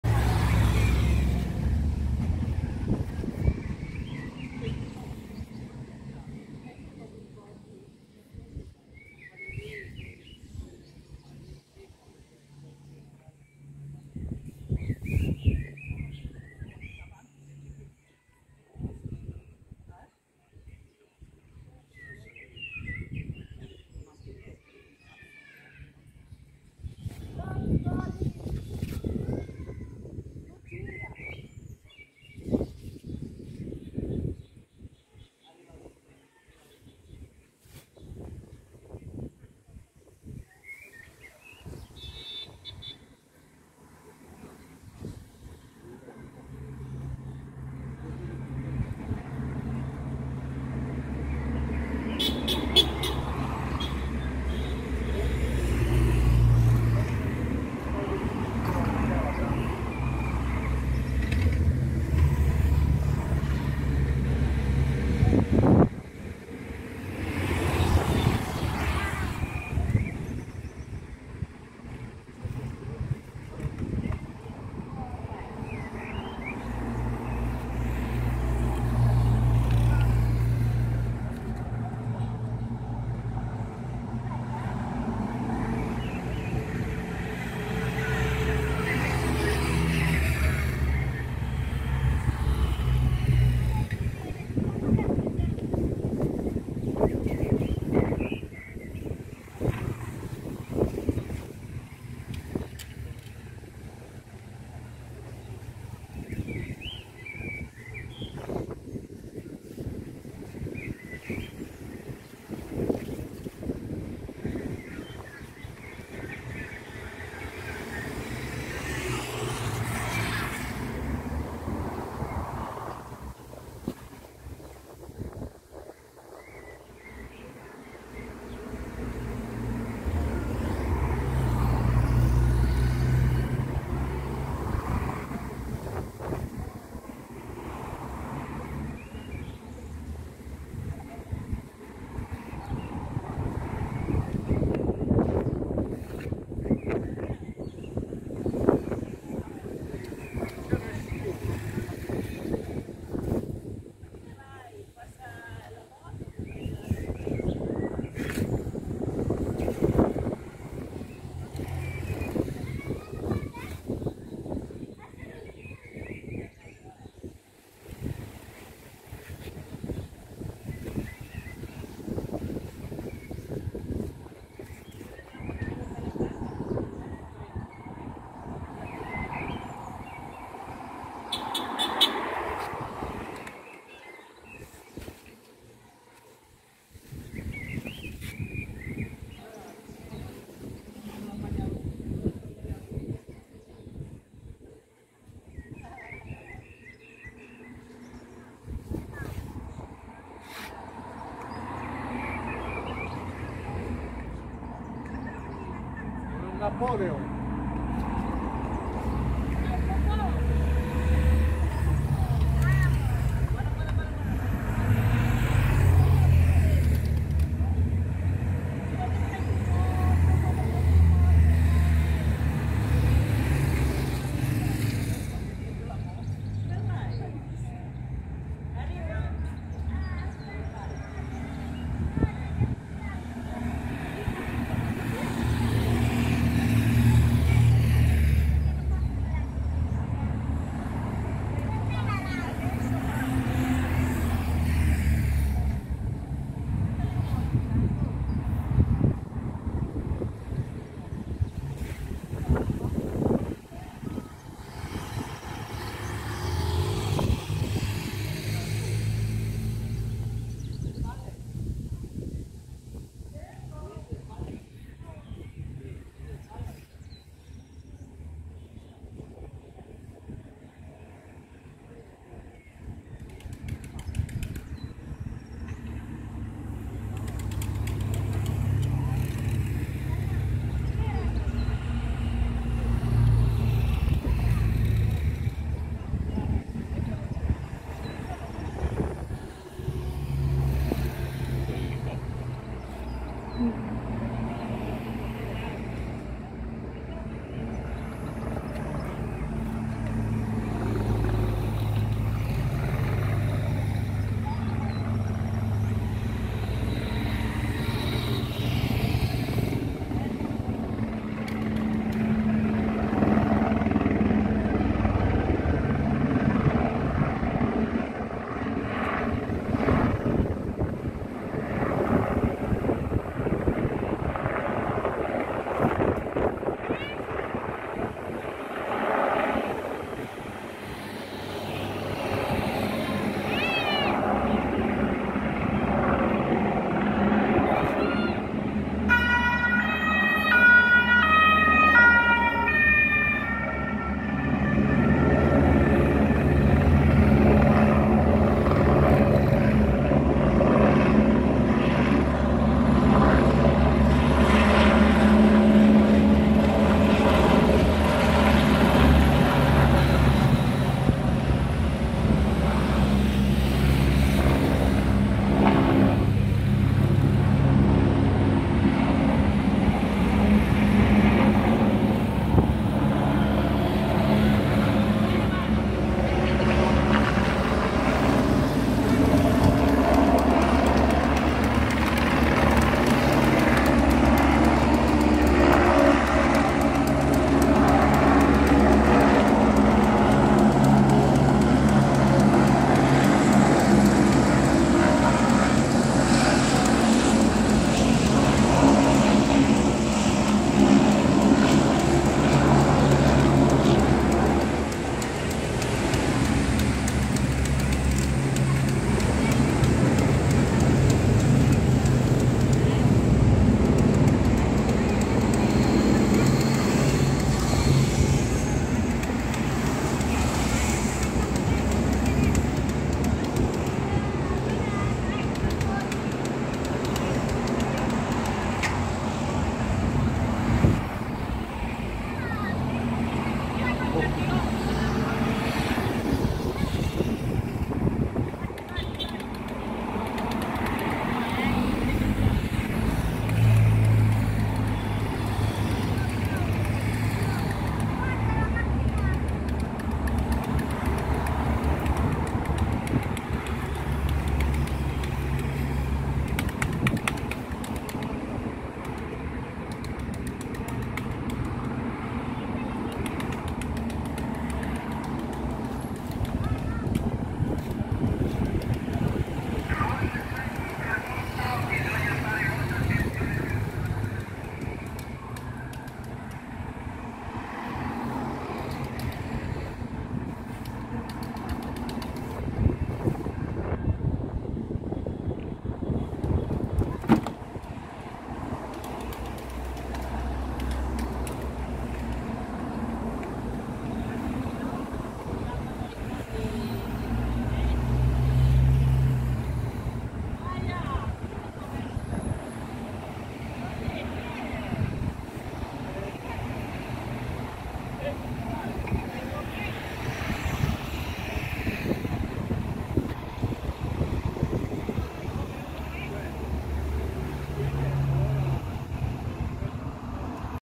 GIRO D'ITALIA TODAY LIVE NATIONWIDE IN FRONT OF OUR HOUSE
GIRO D'ITALIA TODAY IN FRONT OF OUR HOUSE..TELEVISED LIVE NATIONWIDE BY THE HELICOPTER. THIS IS THE 2ND TIME I SAW VERY NEAR LIVE HERE IN OUR PLACE...